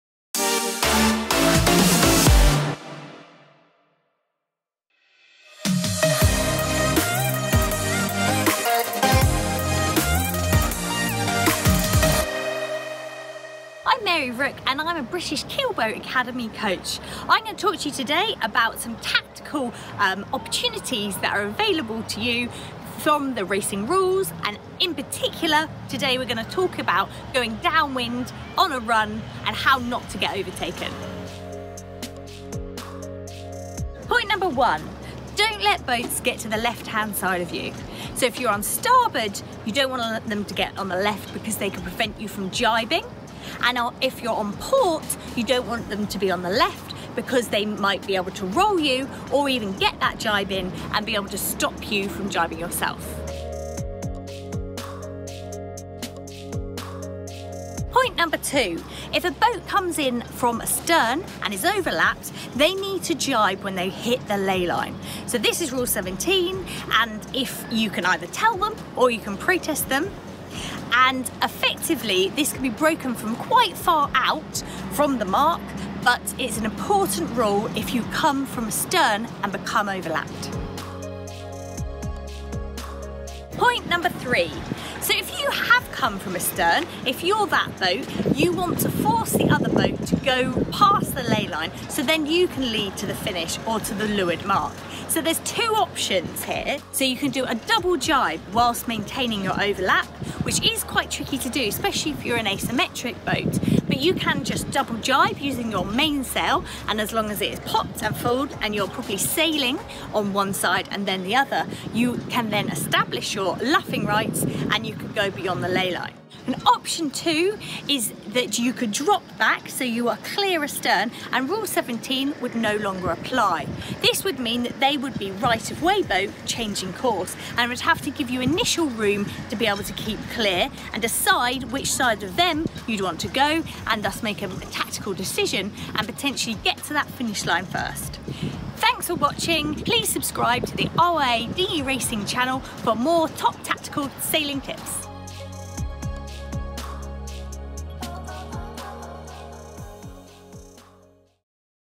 RACING RULES EXPLAINER - EPISODE 3: Downwind Tips
Episode 3 of the Racing Rules Explainer series shows you how to use the Racing Rules of Sailing to improve your downwind skills on the run. Mary Rook from the British Keelboat Academy gives us some top tips.

Animations created using Virtual Regatta.